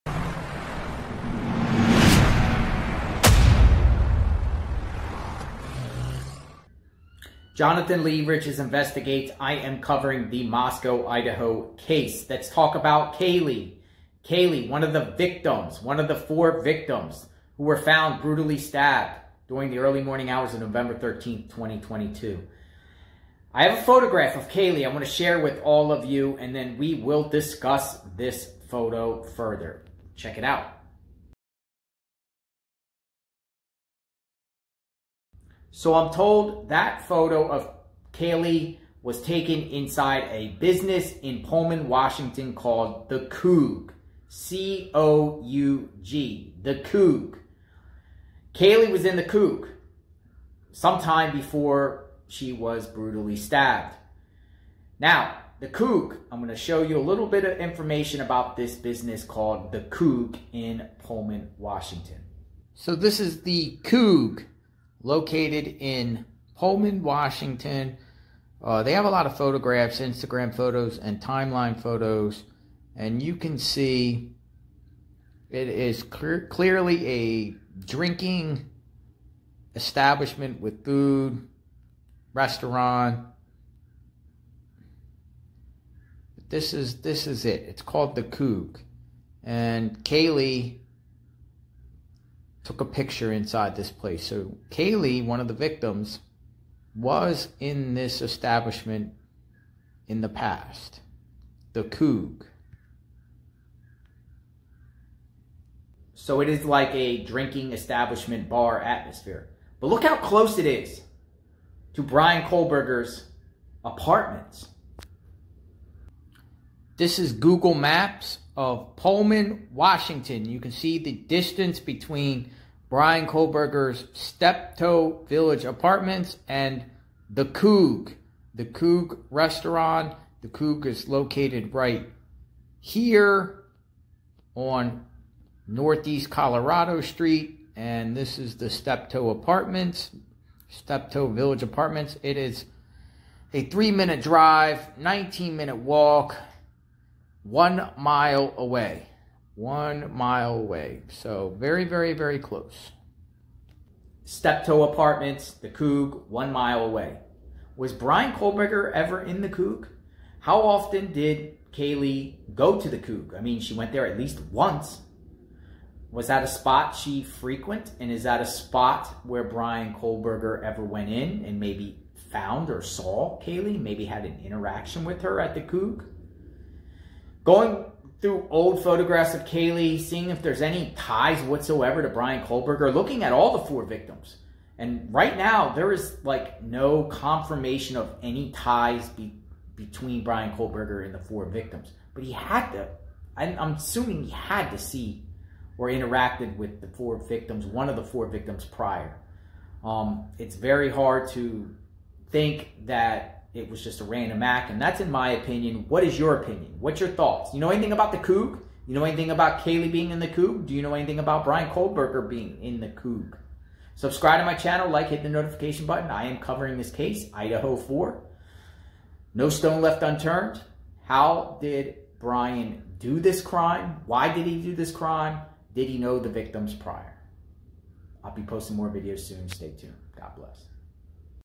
Kaylee Goncalves @ the Coug! Pullman Washington.
I am covering the Bryan Kohberger / Moscow Idaho Case.